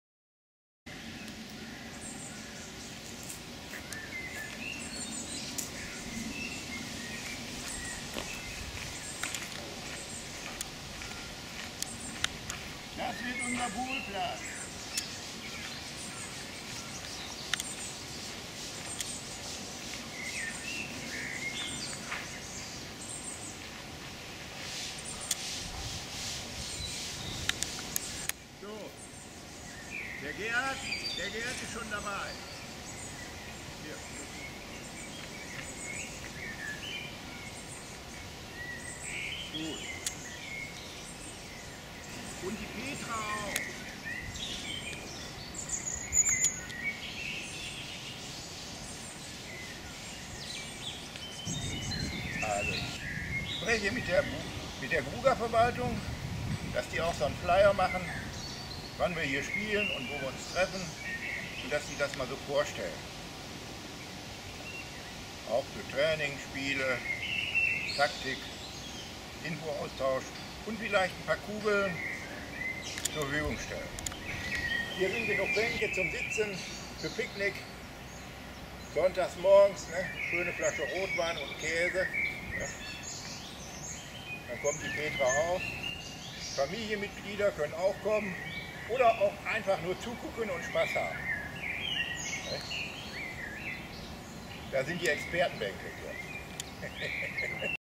Essen Grugapark-Bouleplatz 17.6.2013
Kleine Information über den Bouleplatz im Grugapark von Essen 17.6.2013